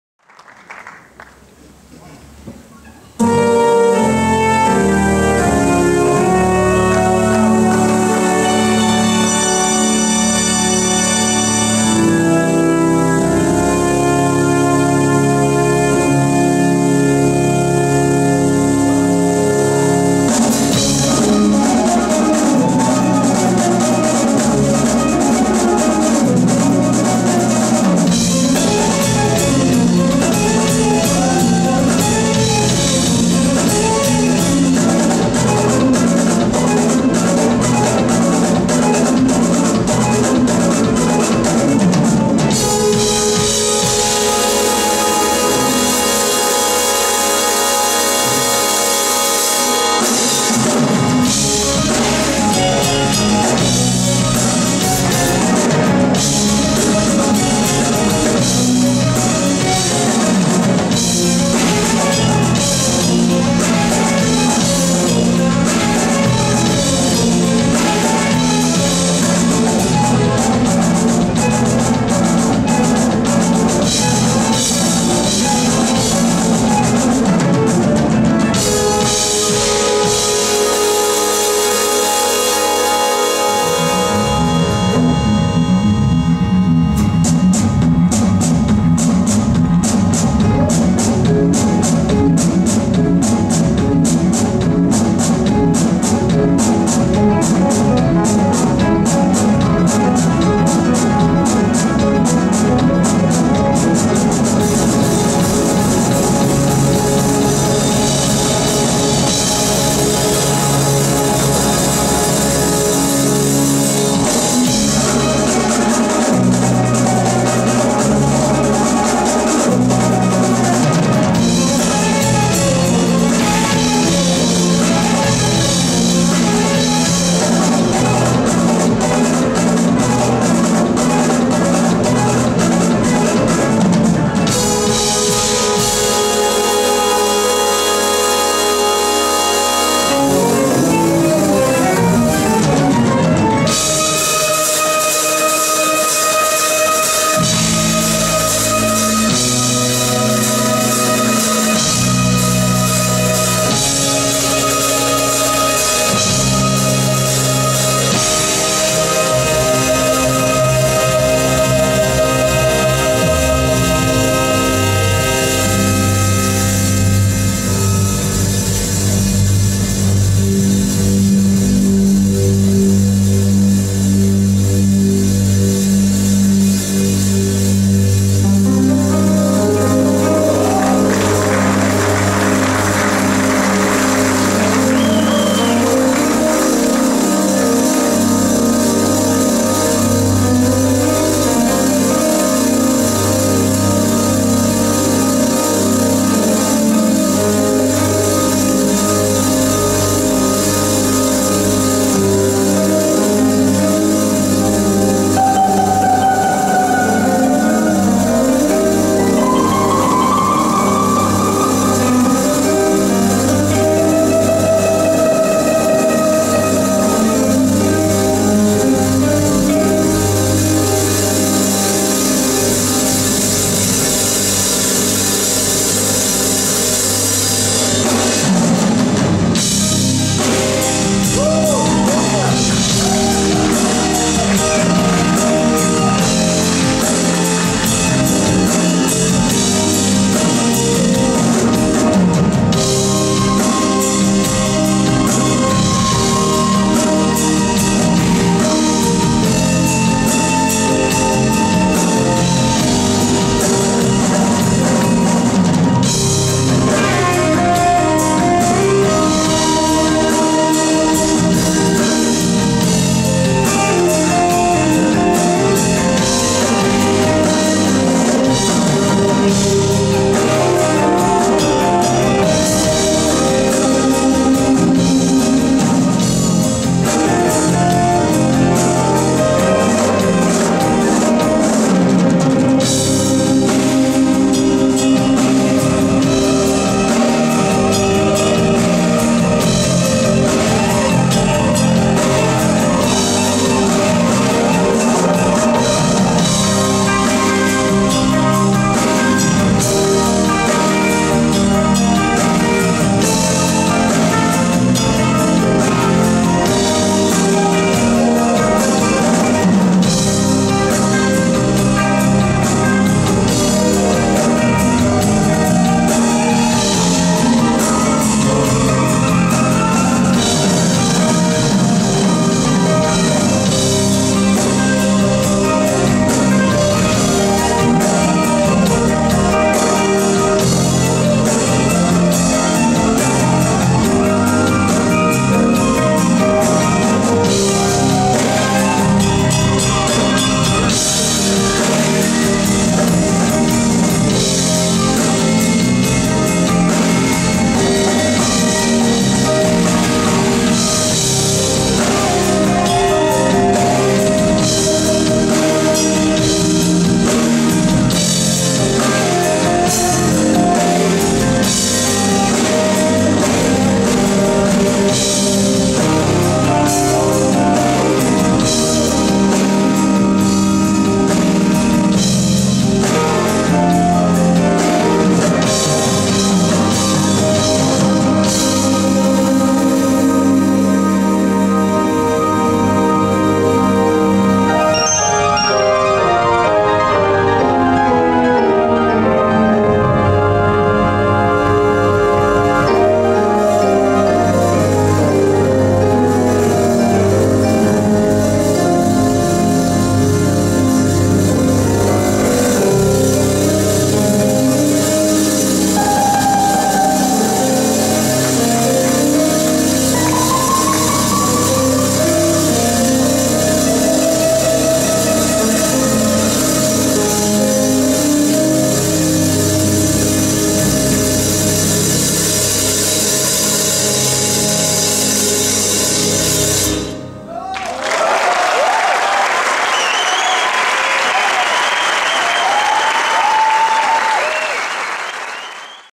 Jean Luc Ponty Antofagasta-Chile 2011.
JLP & HIS BAND will tour in South America in november 2011 with a project called THE ATLANTIC YEARS featuring William Lecomte (keyboards) and members of JLP's American band from the 80s Jamie Glaser (guitar) - Baron Browne (bass) - Rayford Griffin (drums).